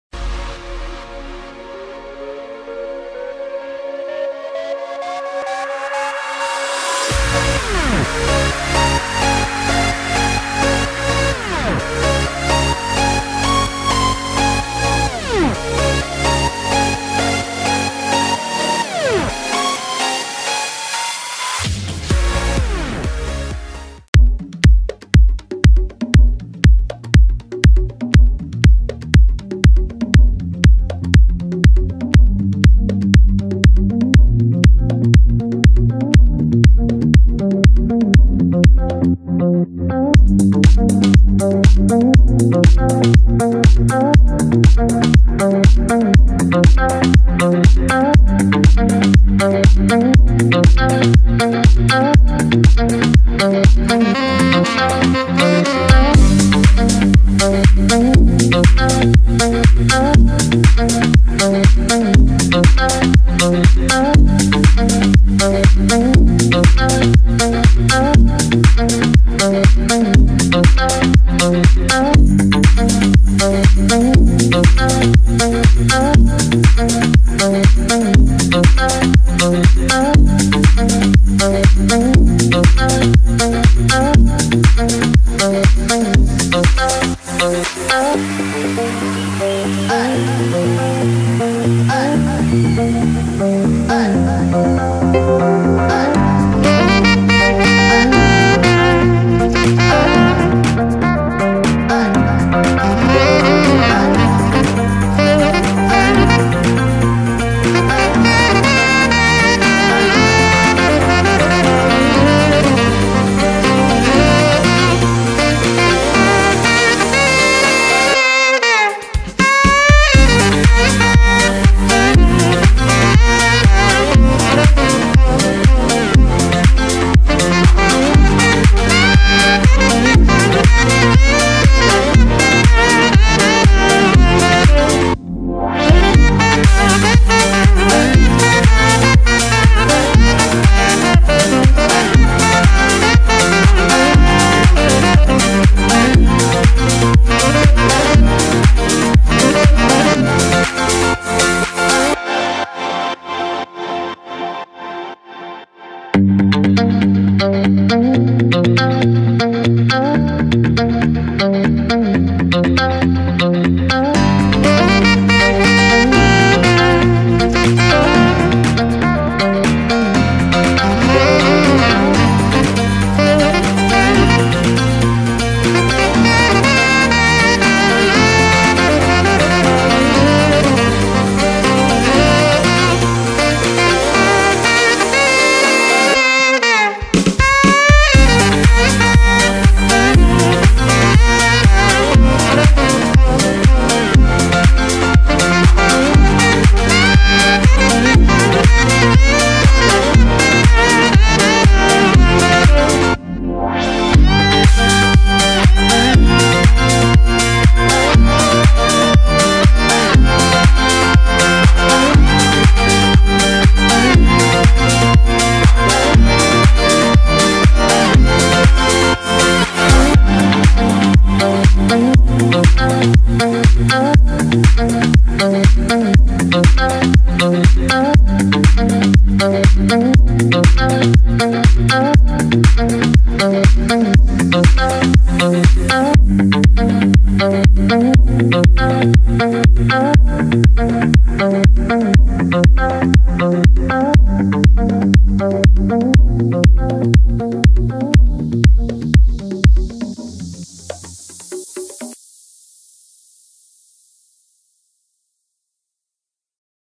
WipeOut FPV Wing - S15 E2 - Backcountry Formation 1
Sunny Sky and two Wings = Fun in the Backcountry!
unfortunately a bit windy
Wing: 2 x WipeOut by Happyflyers